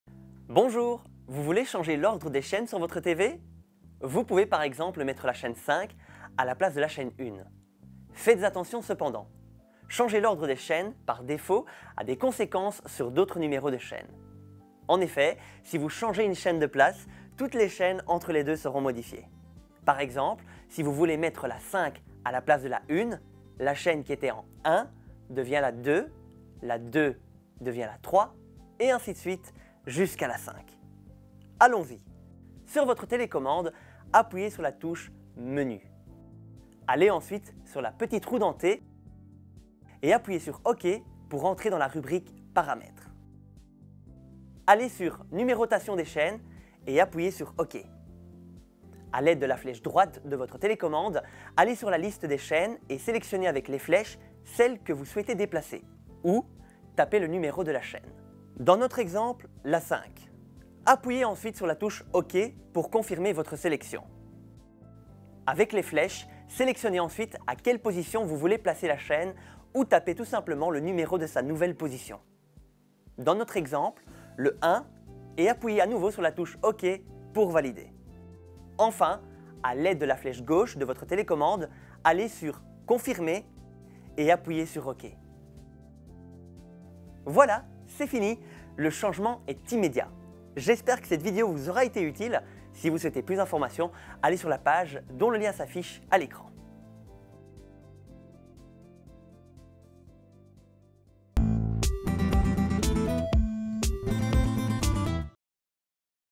Change the order of the tv channels on your Proximus TV Box
In this video, We explain how to change the order of the tv channels on your TV Box and what to take into account when doing this.

Proximus – Think Possible